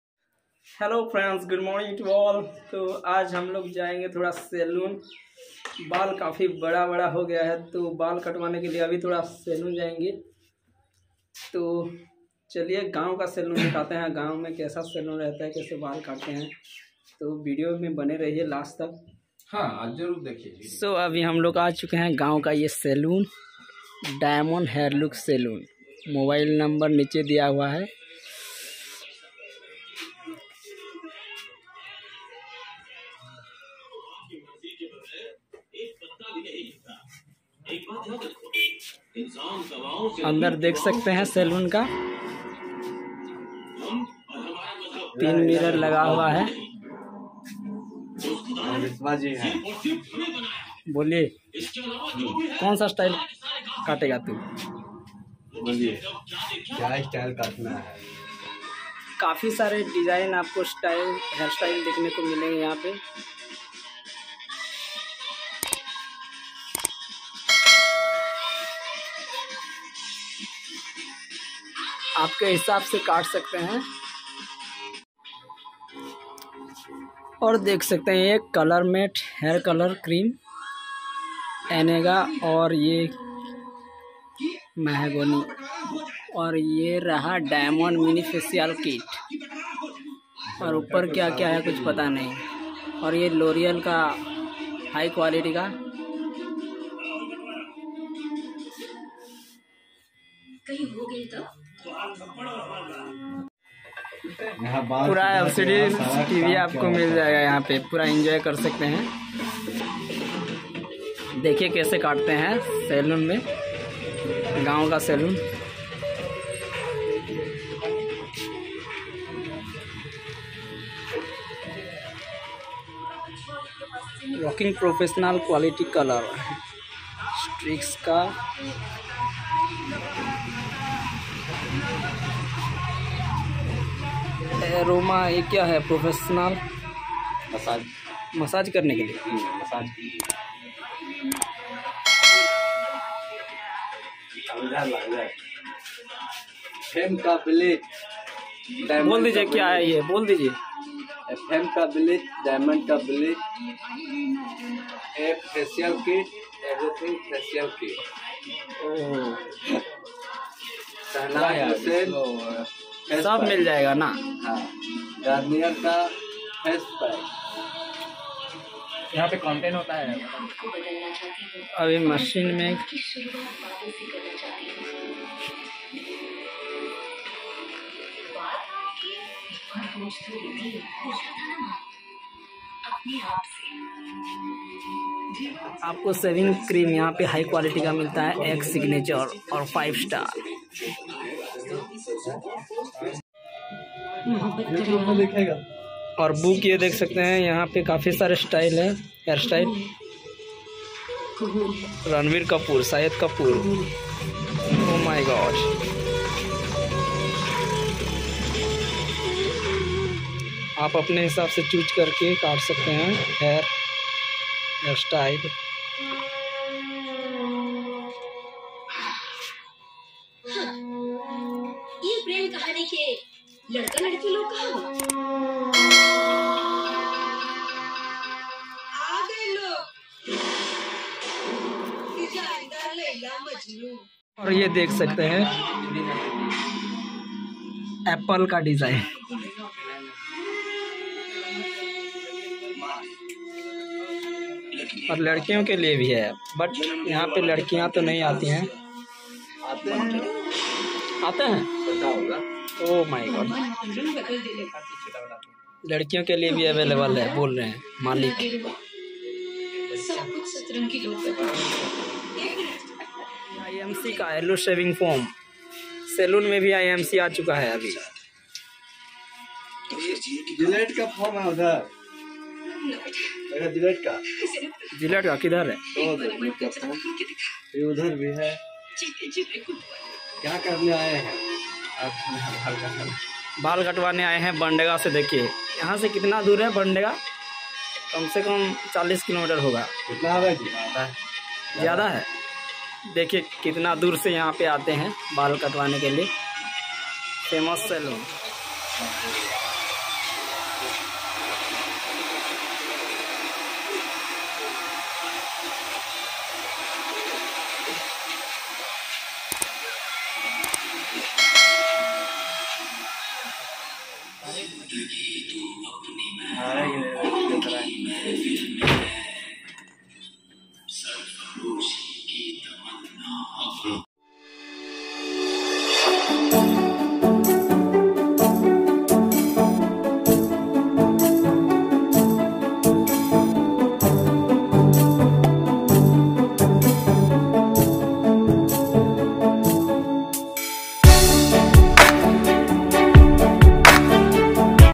Hair Cutting Style// Diamond Hair Look// Village Saloon //Gaon ka saloon
Am Rakesh, And ye Humare gaon ka famous saloon Diamond Hair Look Saloon.Yahan pe har ek design/style k ball katasakte hain. Minimum Price- 50 RS only. Yahan App face wash, massage, and everything is available.
Ajkal gaon main v kafi developed ho Chuka hai.
If you want to come.